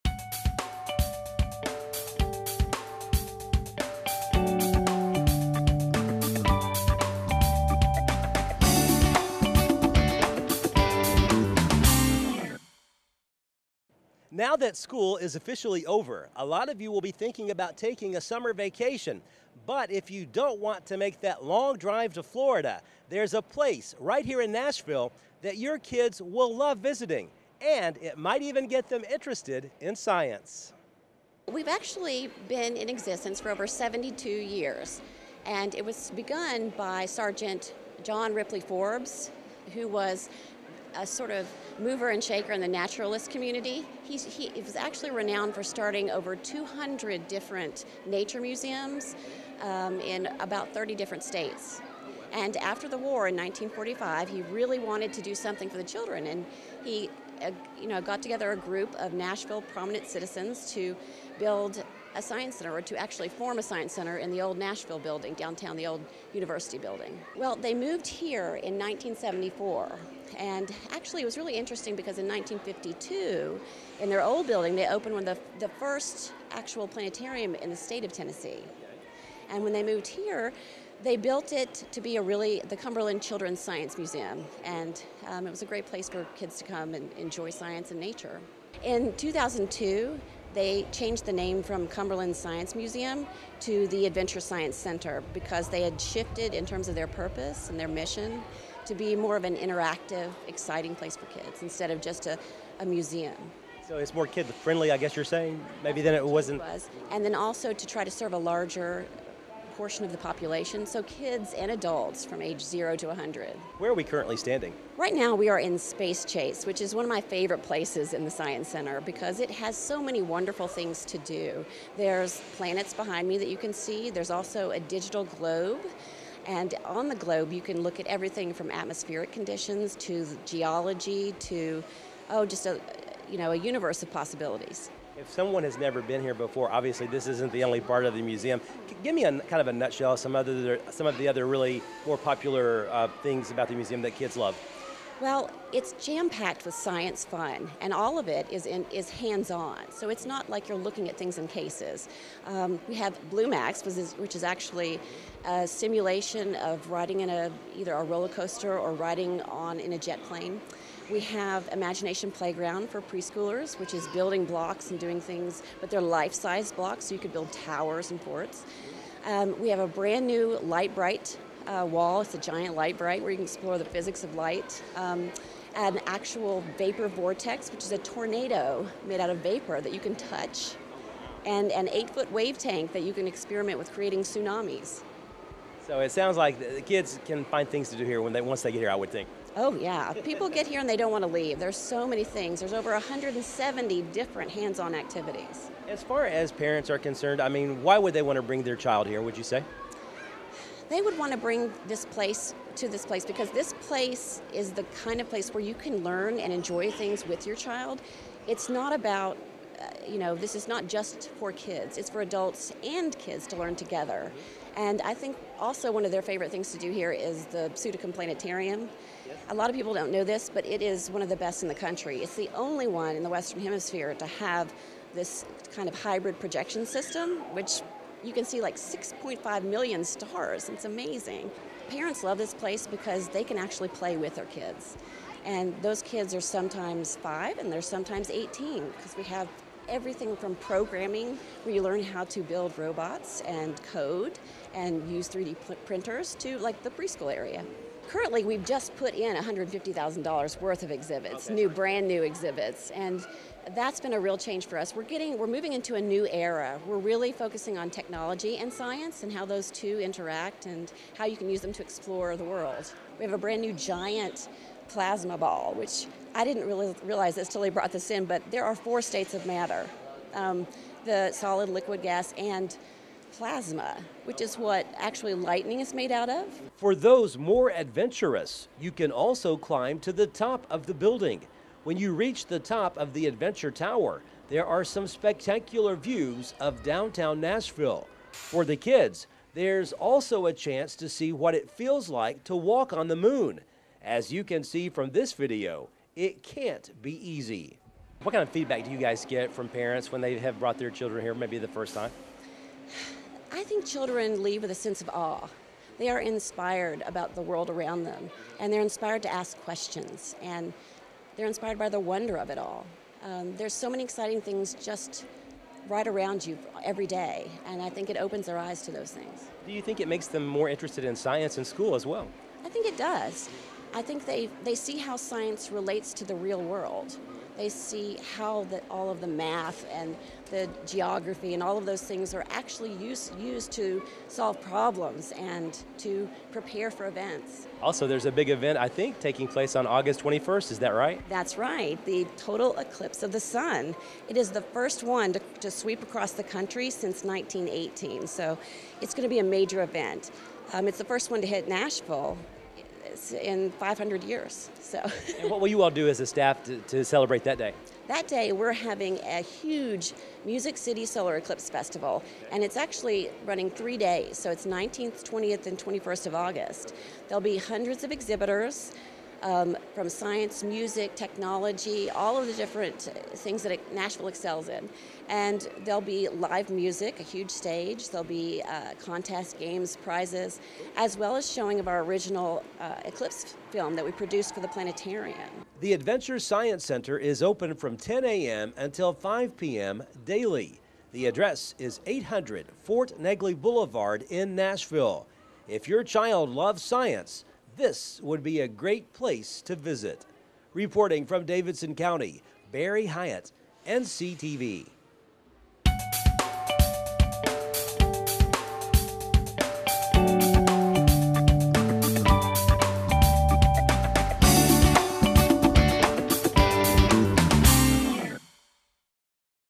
AdventureScienceCenter2017
This week on NCTV, we have an adventure as we visit the Adventure Science Center!